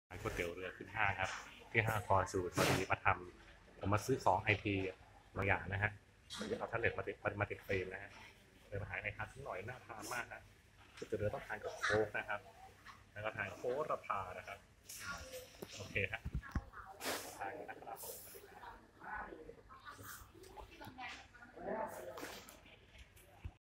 พอดีวันนี้นำเครื่องแท็ปเล็ตมาติดฟิลม์หน้าจอและมาซื้อ Soft case ที่ห้างฟอร์จูนทาว์
พอดีวันนี้นำเครื่องแท็ปเล็ตมาติดฟิลม์หน้าจอและมาซื้อ Soft case ที่ห้างฟอร์จูนทาว์ เสร็จภาระกิจแล้วทานก๋วยเตี๋ยวเรือร้อนๆอร่อยๆดีกว่าครับ